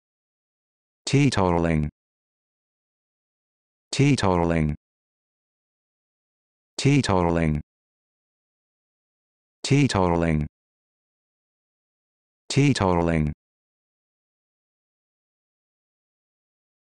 How to Pronounce Teetotalling
This video shows you how to pronounce Teetotalling